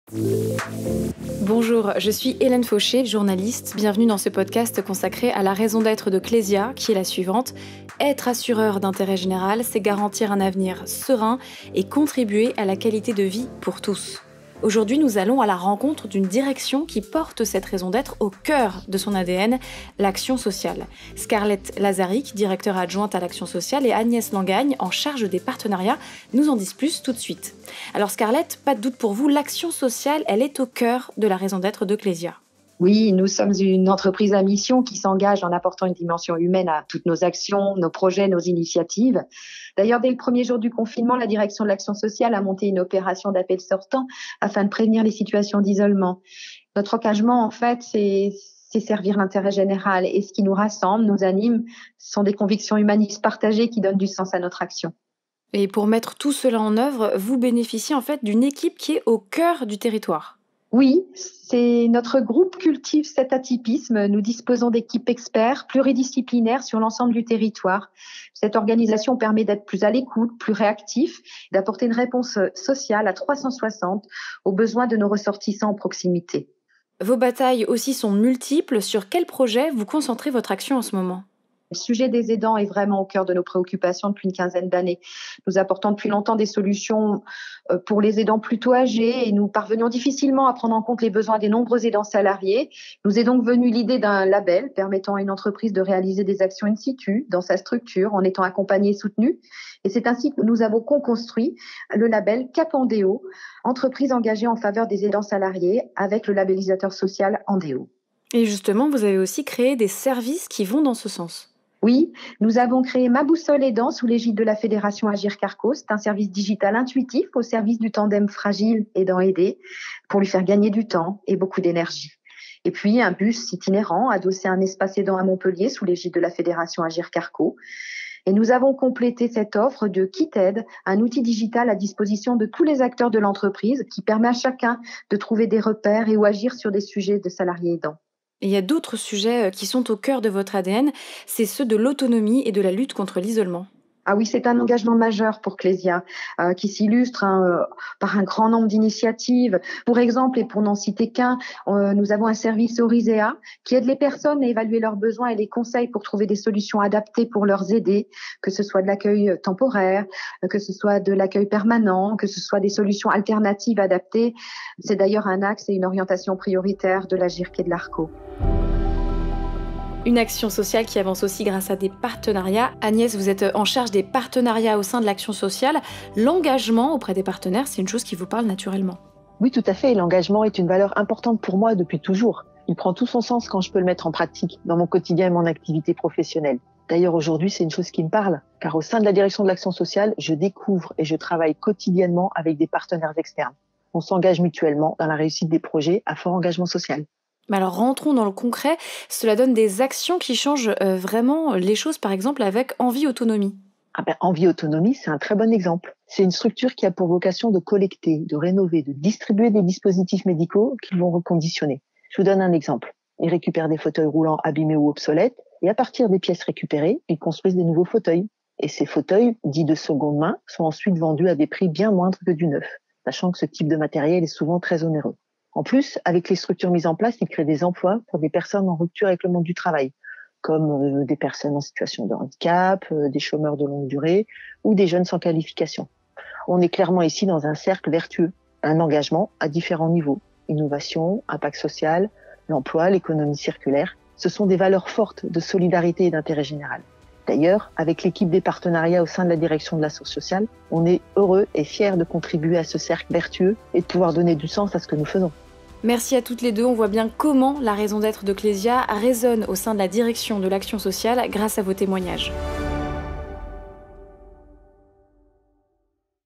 Notre Raison d'Être vue par Scarlett Lazaric et Agnès Lengagne de l'Action sociale KLESIA
Scarlett Lazaric et Agnès Lengagne de l'Action sociale KLESIA partagent leur vision de notre raison d’être et les actions qui l’incarnent.